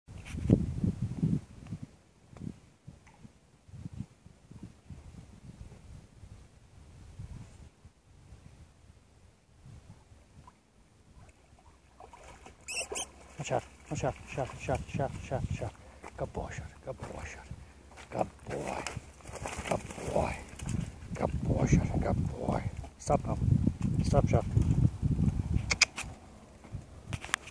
Springer Spaniel hunting dog
Dog was trained on woodcock and pheasant and was retrieving at seven months end of last season.  For  hunting  only not sold  as pet.